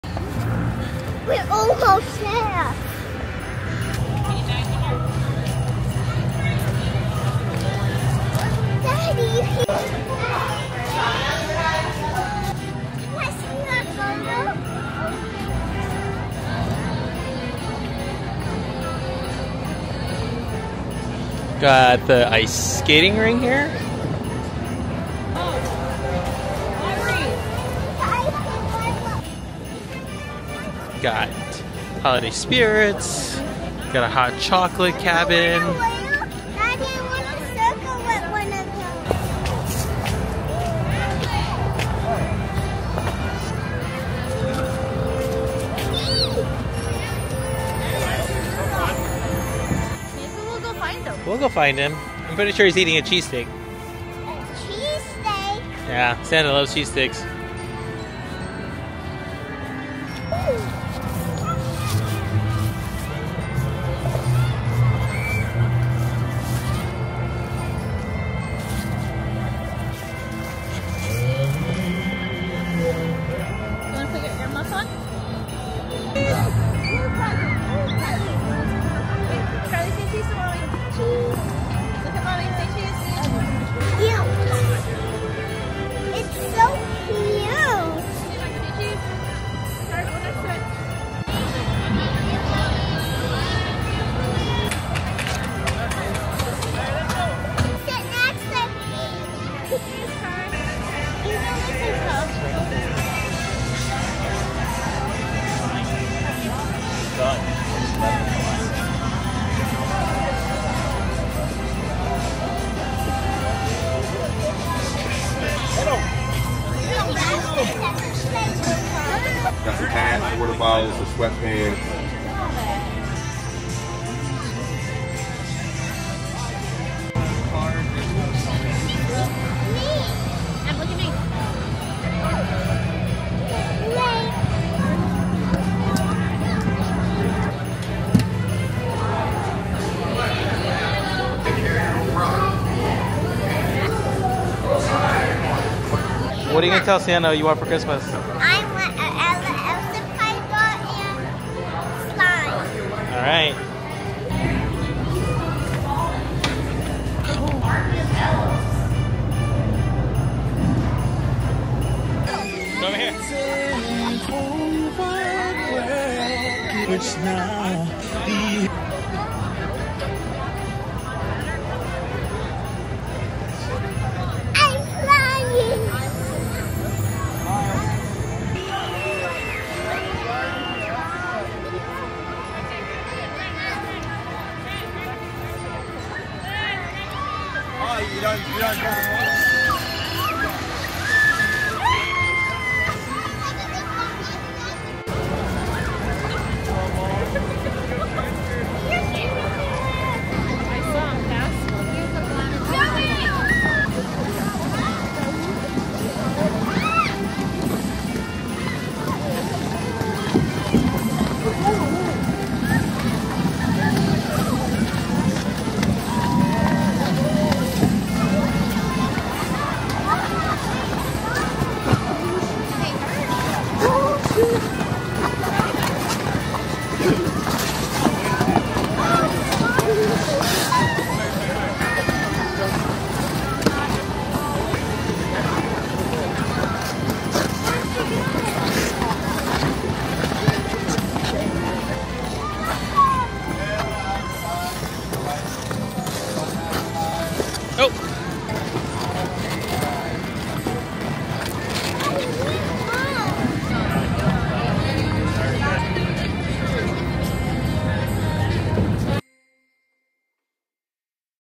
Tinseltown Holiday Spectacular 2023 at FDR Park, Philadelphia 🎄 | Tour 4K
We got free tickets from our neighbors to Tinseltown! So we decided to jot over to Tinseltown in FDR park on a Friday evening.

Happy Holidays!

📚 Chapters
00:06 Entrance
00:43 Light Trail
01:40 Mistletoe Marketplace
02:05 Gritty Santa
02:17 Santa's Shop
02:23 Mrs Claus Book Reading
02:44 B101 Dance Area
03:00 Ice Slide
03:22 Ice Trail Tour